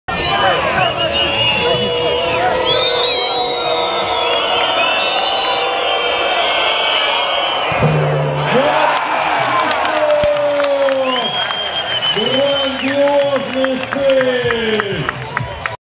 Рафинья бьет штрафной, как пенальти - Чужков отбивает
Штрафной в ворота Ротор исполянет бразилец Рафинья из Локомотива, но пробить Чужкова не смог. Второй полуфинальный матч Суперфинала чемпионата России